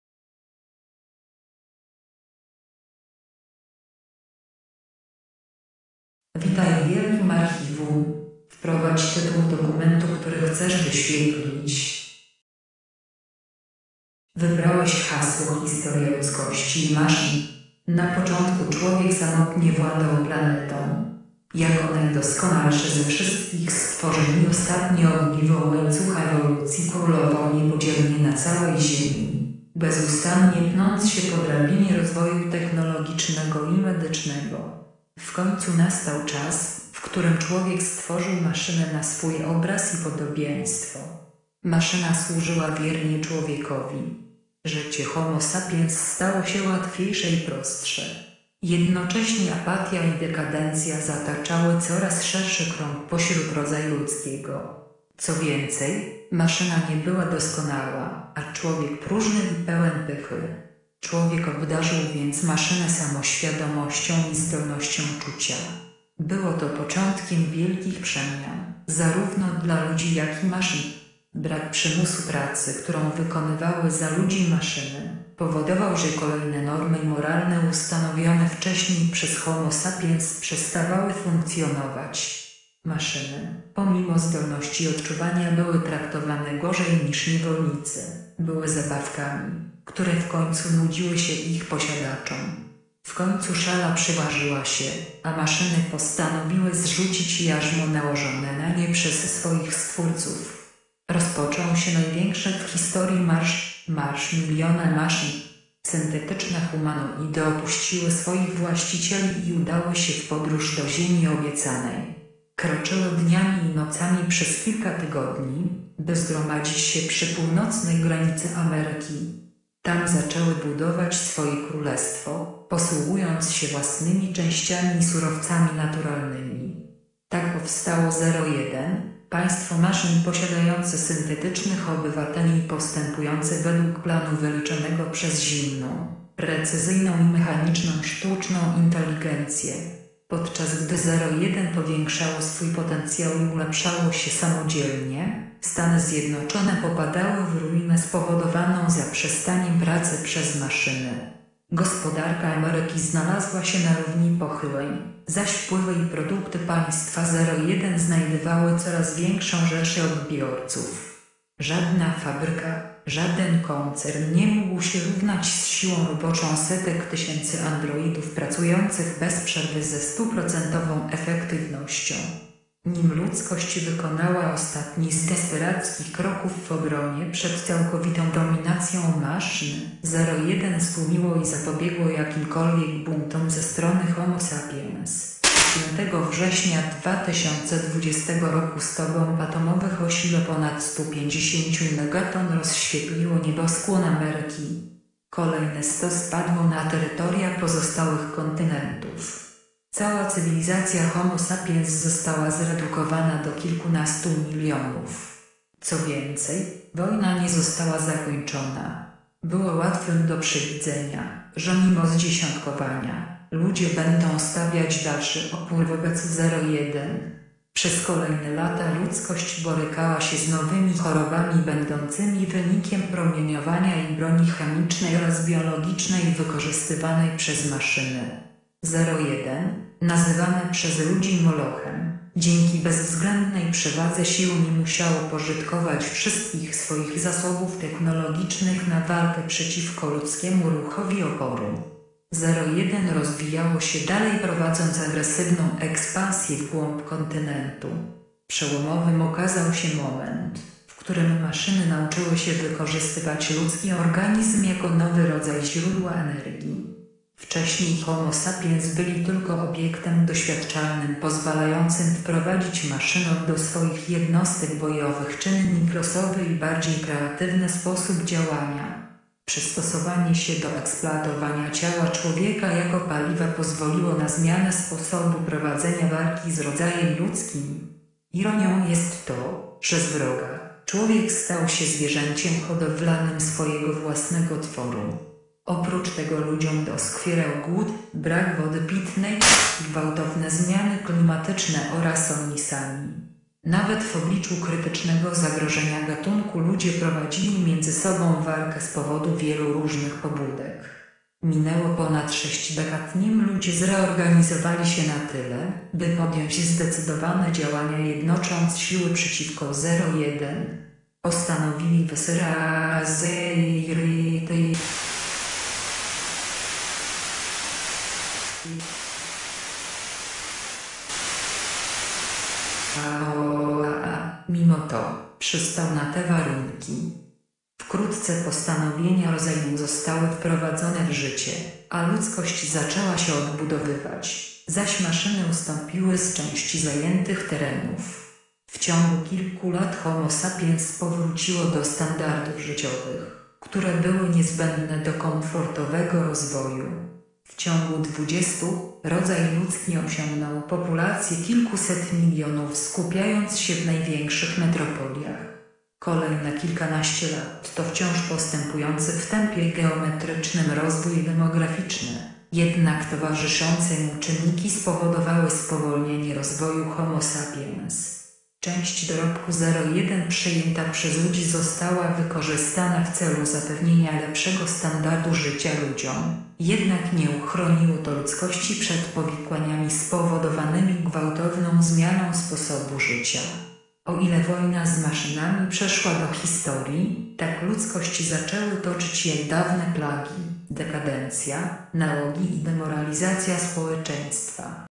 Kronika ludzi i maszyn
Skrócony zapis wydarzeń od powstania sztucznej inteligencji i wybuchu Trzeciej Wojny Światowej (2020 rok) do rekonstrukcji cywilizacji w XXII wieku.